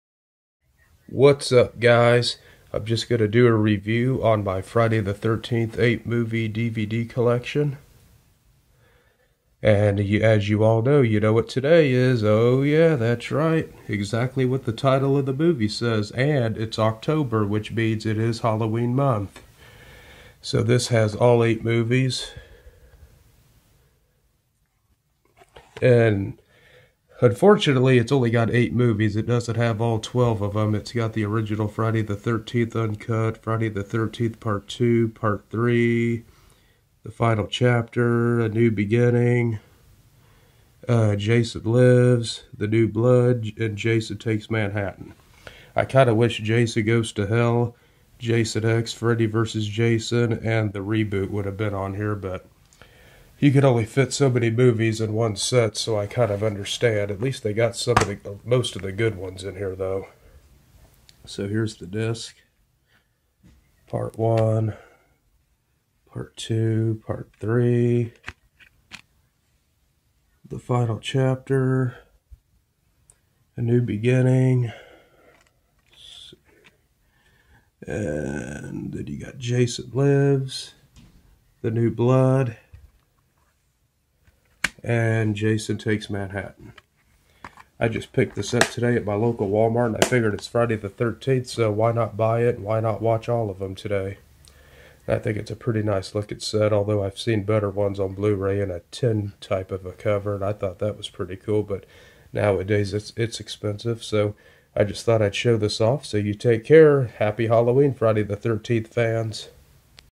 Friday The 13th 8 Movie Collection DVD
I bought this today at my local Wal-Mart
worth the buy especially since it is...what the title of the movie says.